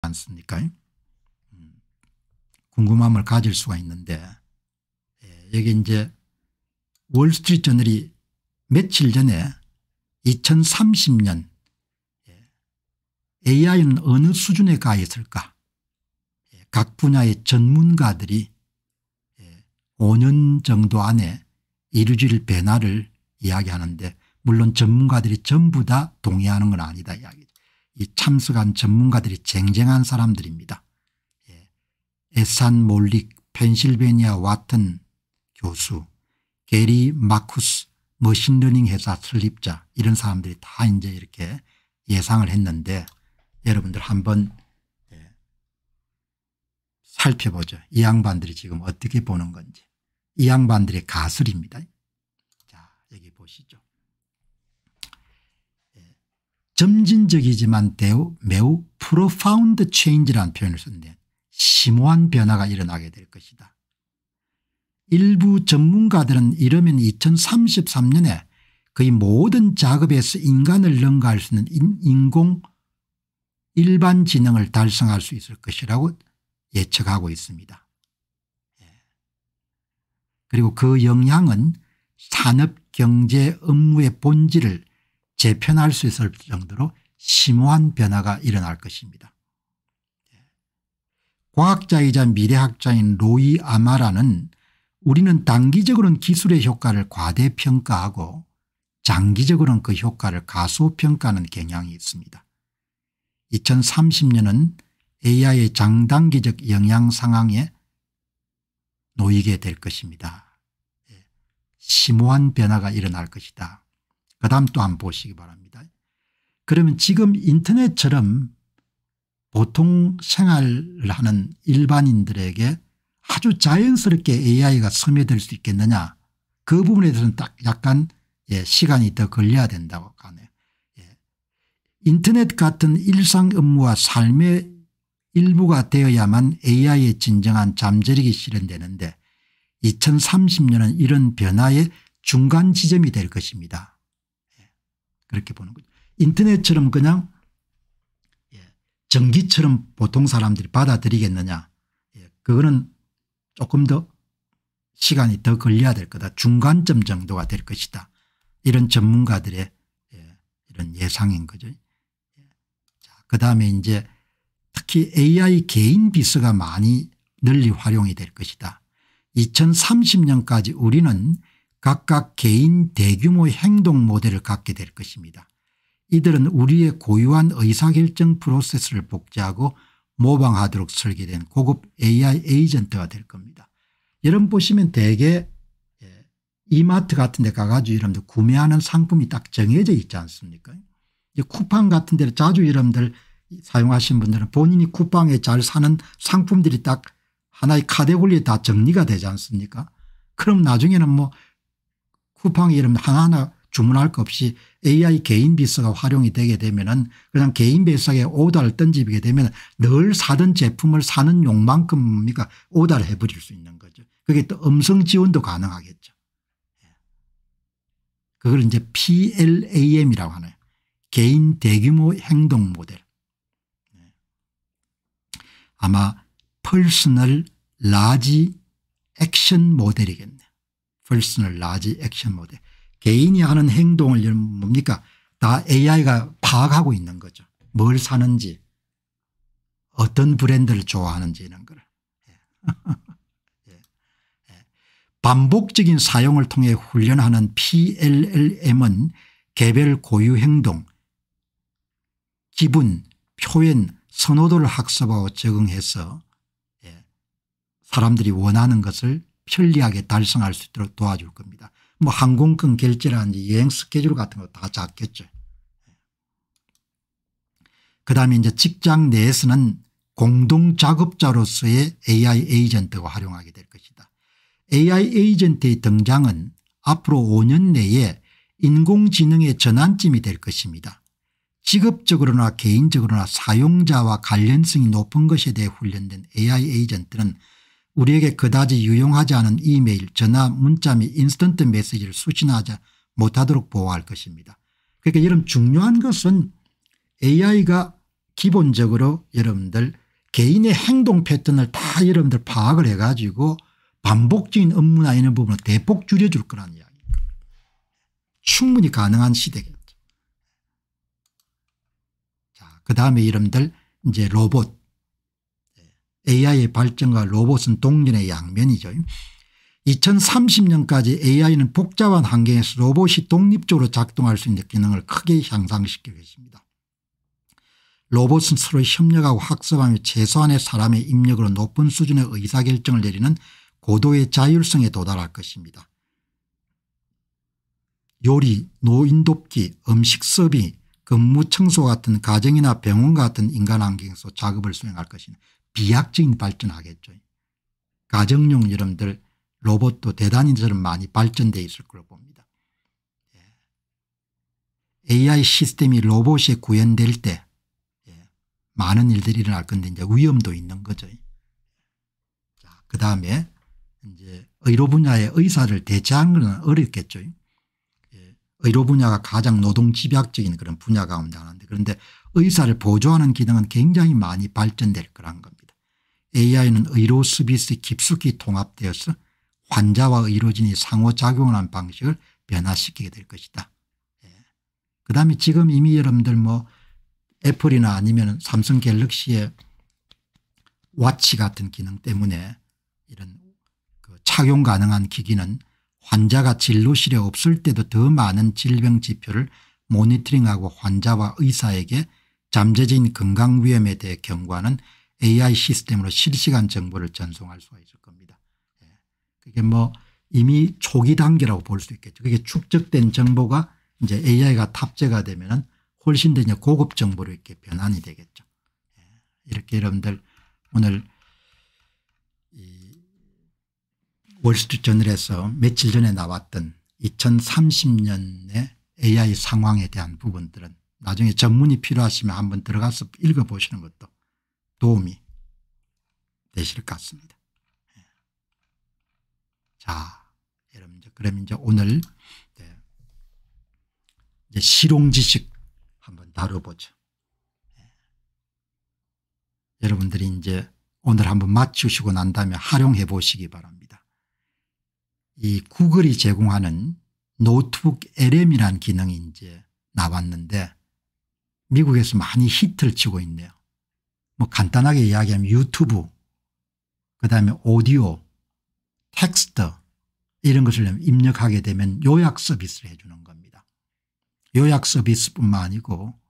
2030년 되면 / 미리 가보는 미래 / AI가 가져올 변화 전망 [공병호TV]
Bitcoin : 3MTW849AkP2ftLeSK2teZHMmzLu7Z83s9U